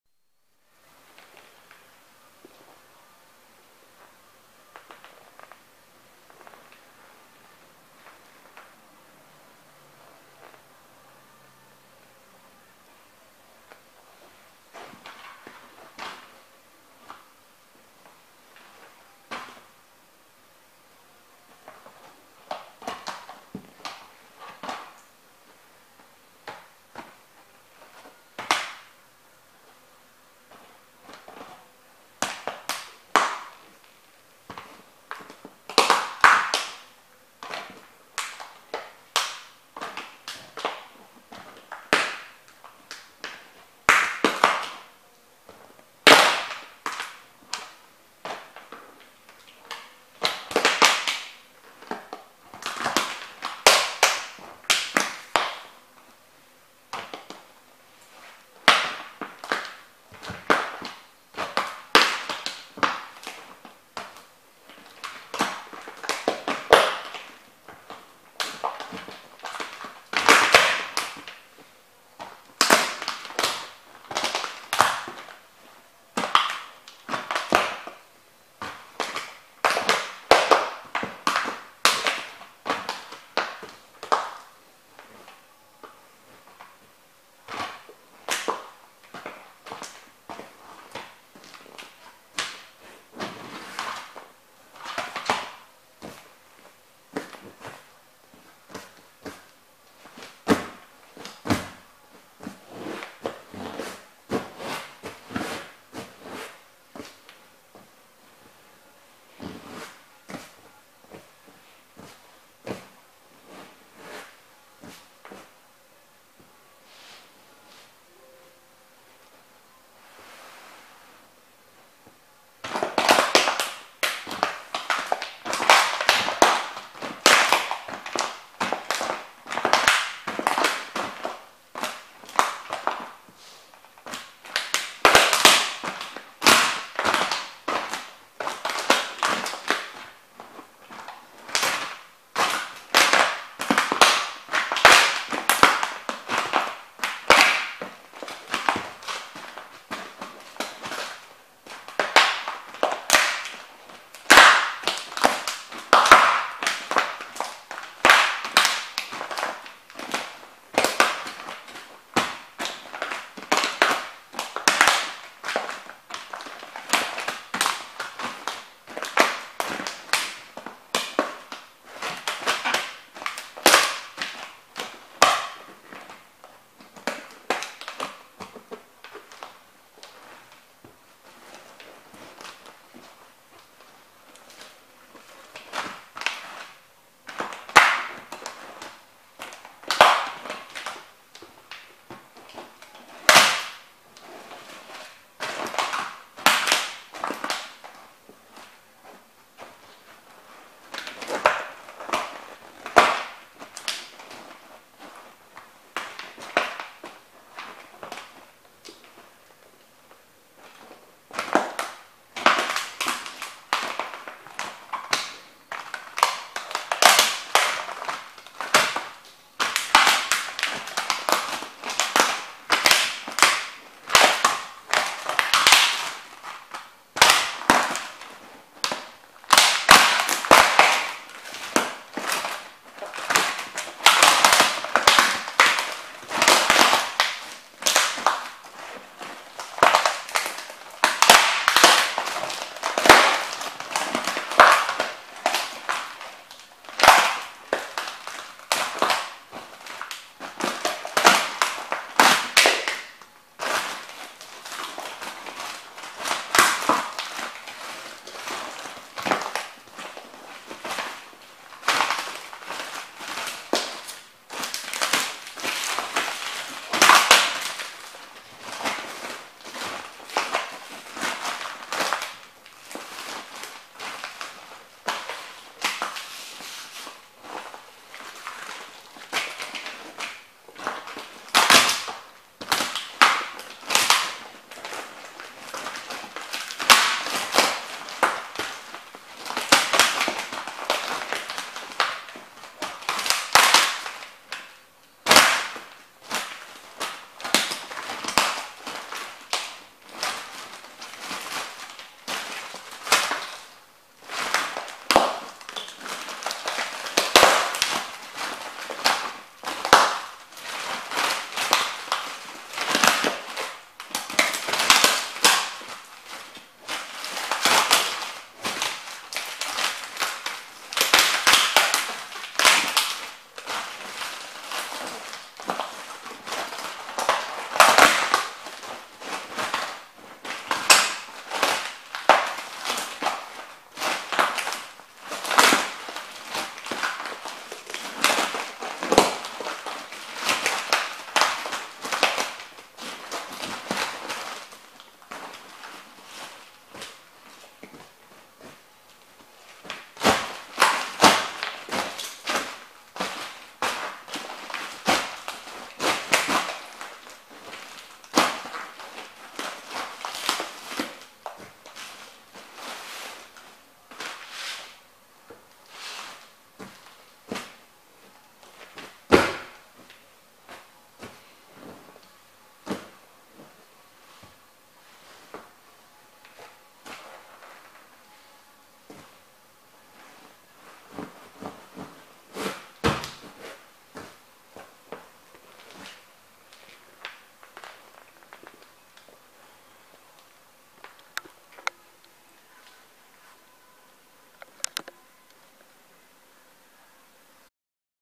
Dr Martens Steel Toe Boots stomp, trample and destroy DVD boxes on leather cushions / pillows
I destroyed some DVD boxes on top of these donated pillows with my new Dr Martens Boots.